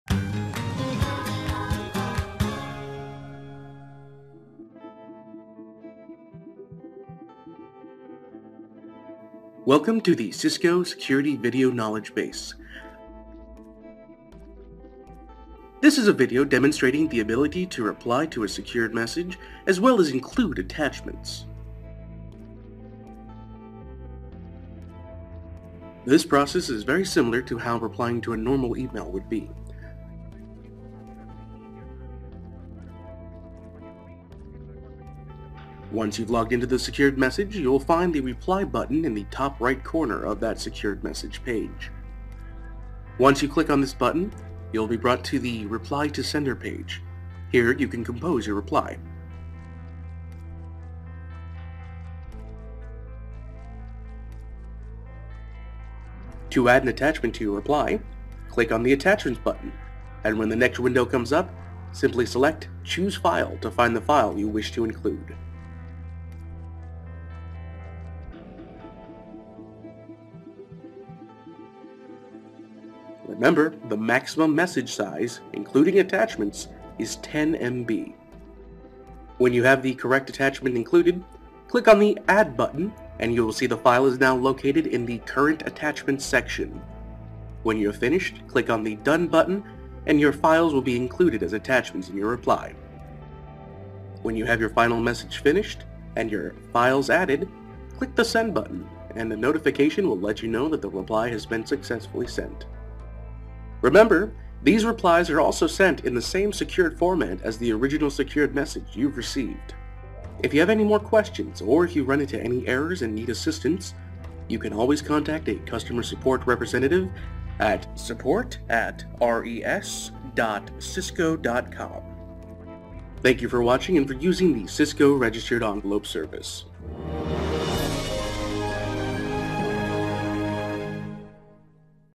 CRES: Features: Replying and Attachments
How to reply to a Cisco Registered Envelope Service message and how to handle attachments in your CRES message.